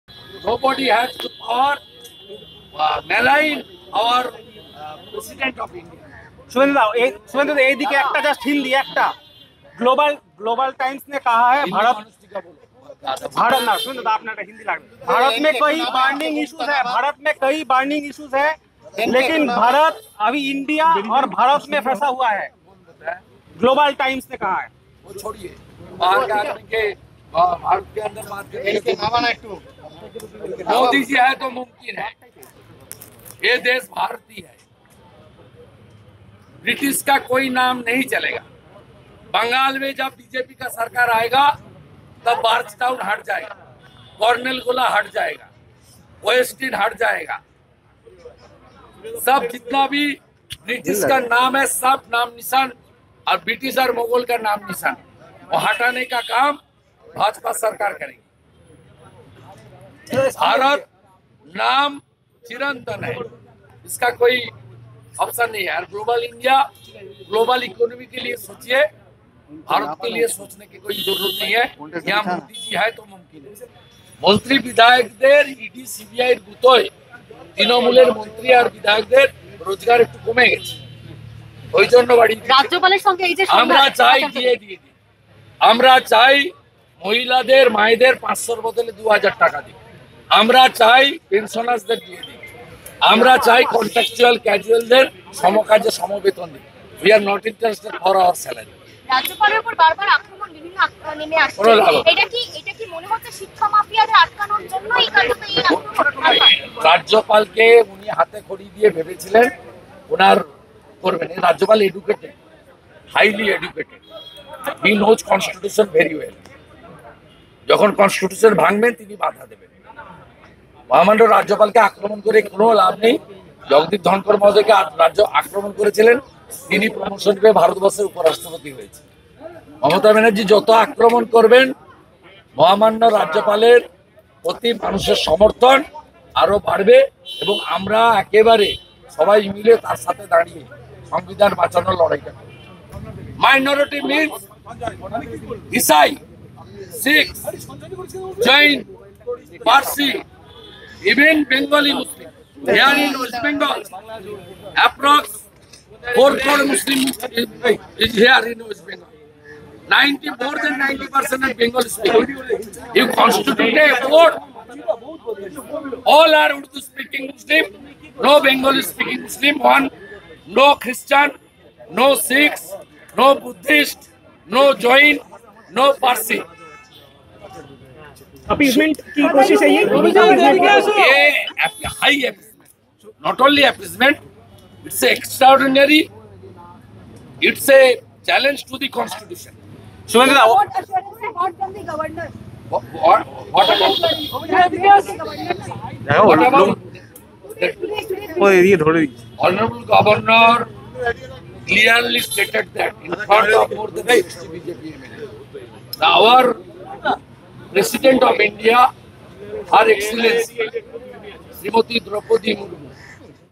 Suvendu Adhikari: বাংলায় বিজেপি ক্ষমতায় এলে ইংরেজ-মুঘলের নাম ও চিহ্ন মুছে ফেলা হবে
Suvendu Adhikari Slams Govt: বাংলায় বিজেপি ক্ষমতায় এলে ইংরেজ ও মুঘলের যাবতীয় নাম ও চিহ্ন মুছে ফেলা হবে বলে দাবি করলেন বিরোধী দলনেতা শুভেন্দু অধিকারী ৷